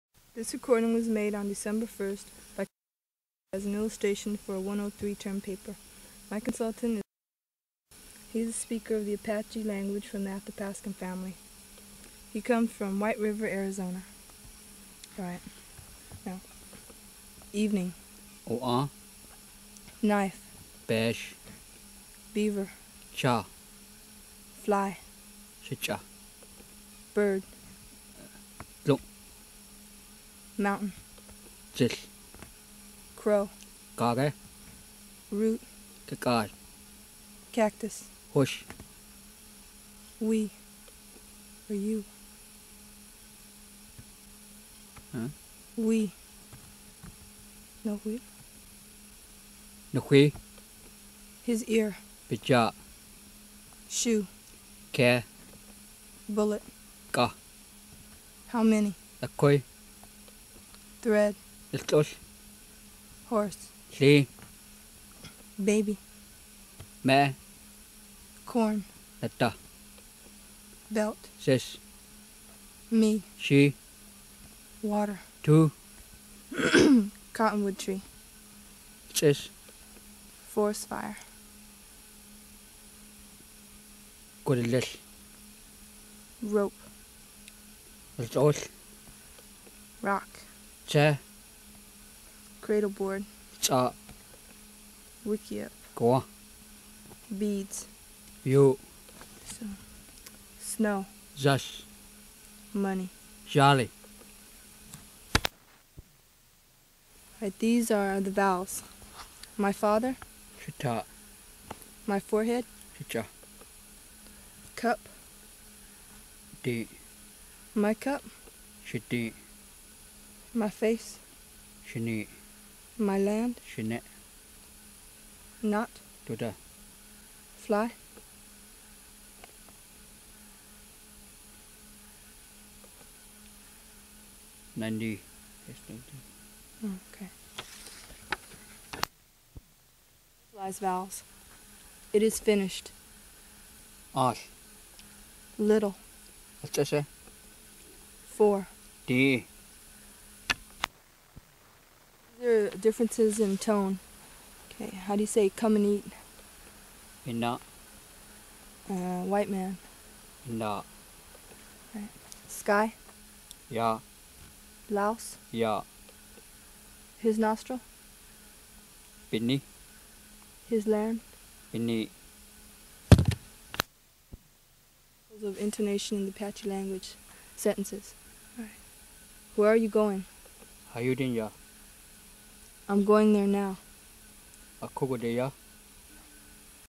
Western Apache audio: apw_word-list_1980_01
Western Apache (apw) audio: apw_word-list_1980_01
Language family: Eyak-Athabaskan; Athabaskan; Southern Athabaskan (Apachean);
University Of California, Los Angeles

Description: Language Apache (Western).  apw_word-list_1980_01.html, entries: 1 - 50

Contributor: N/A
UCLA Phonetics Archive